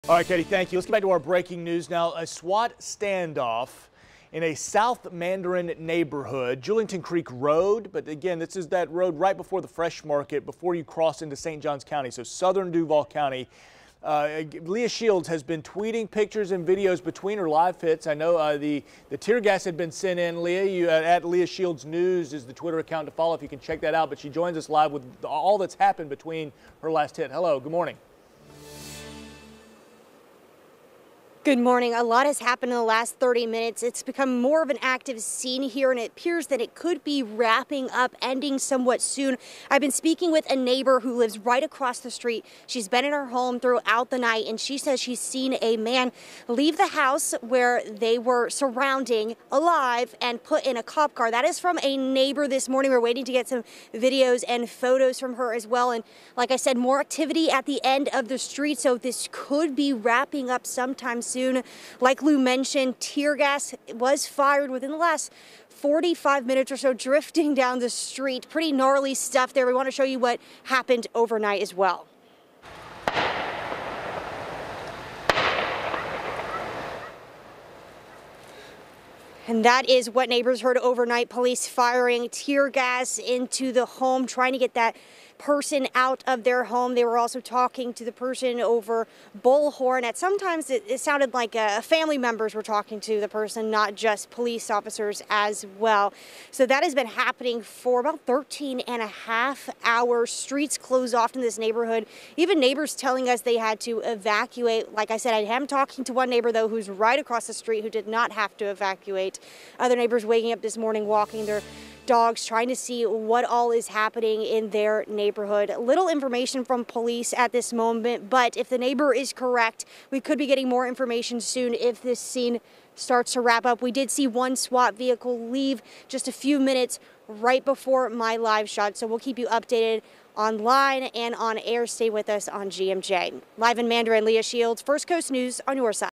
SWAT standoff in Mandarin area shuts down roadway for hours
Several police units with the Jacksonville Sheriff's Office responded to the scene around 4:30 p.m. Monday afternoon.